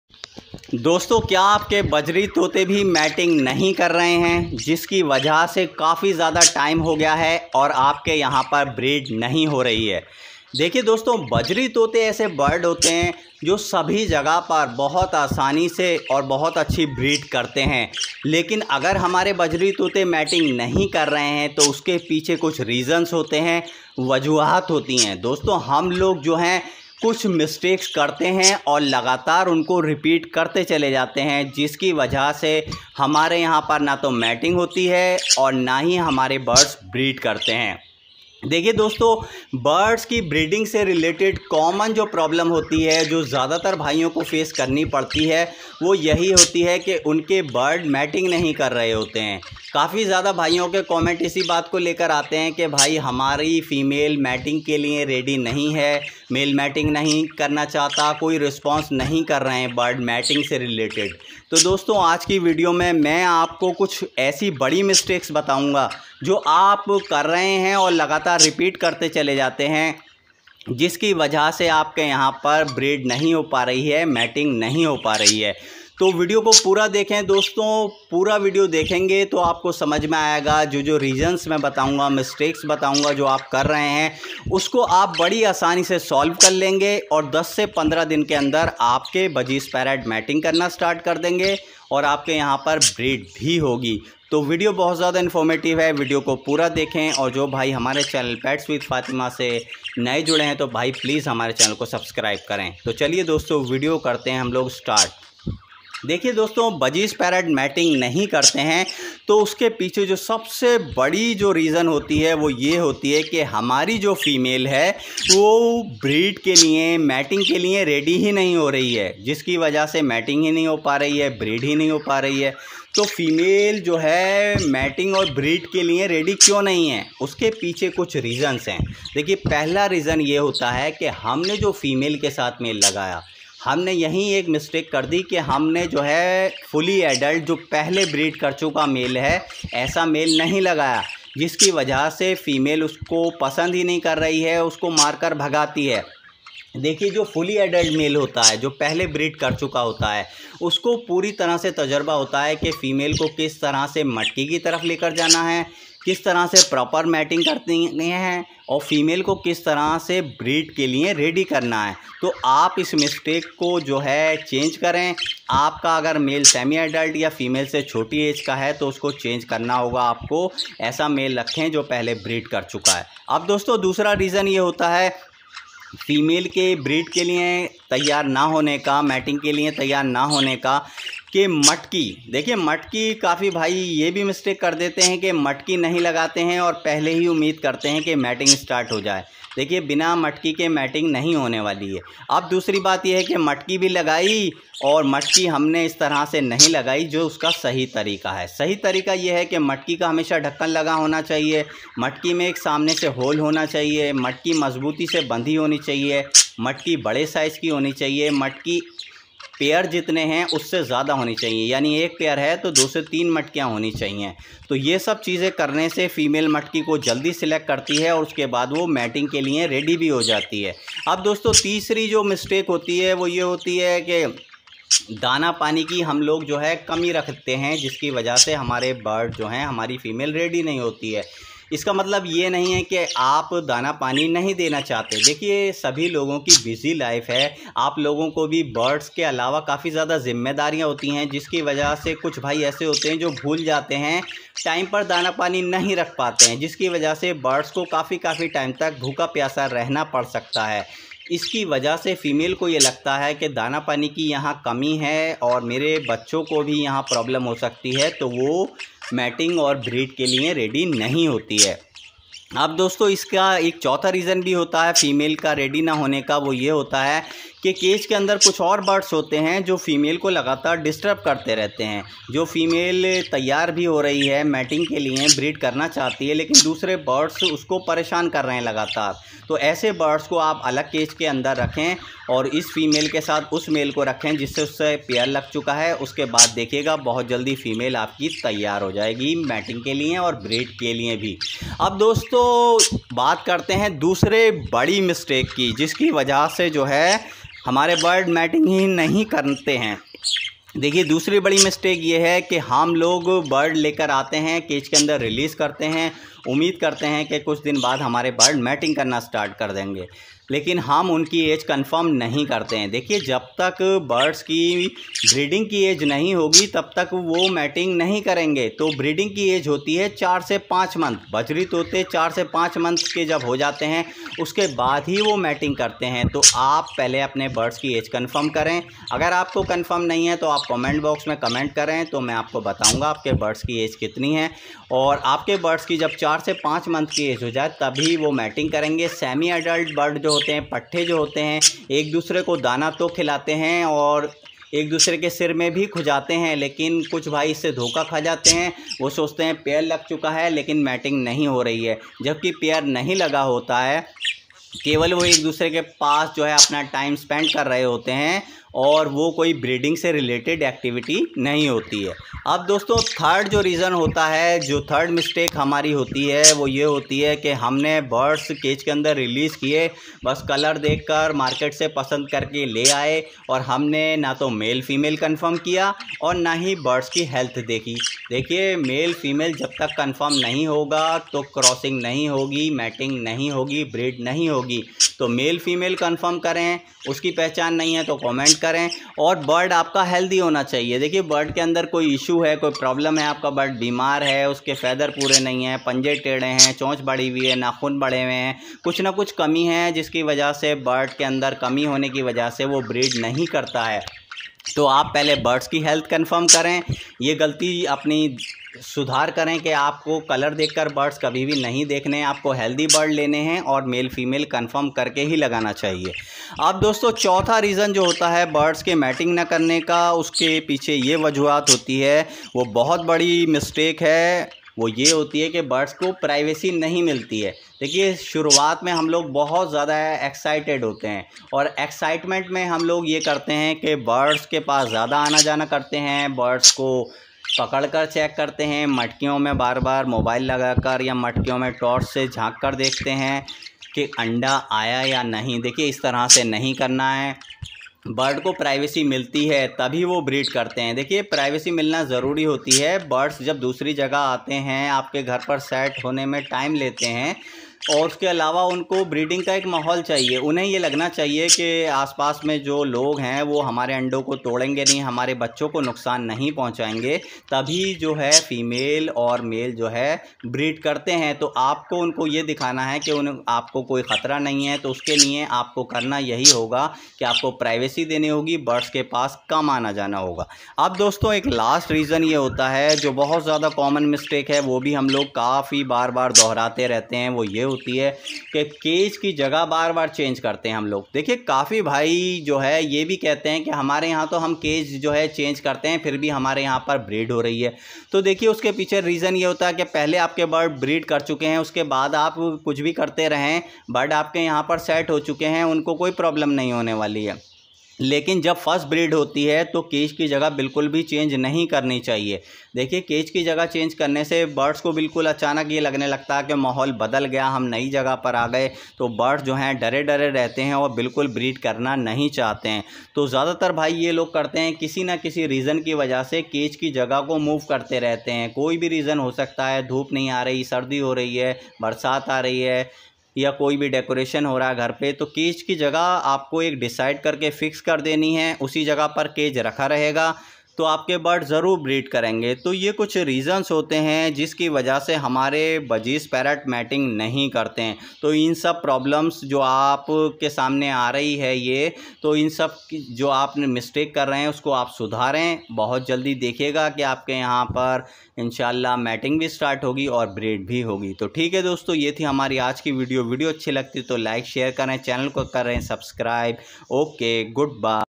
Bajri Tote Mating Nahi Karte Uskai 5 Main Reasons | Budgies Mating Formula 100 % Result
Hi guys aaj ki video mai humne bataya hai kai 5 badi wajah jiski wajah se bajri tote mating yani cross Nahi Karte or iska Solution bhi bataya hai to aap video ko pura zarror dekhai or channel ko subscribe kare.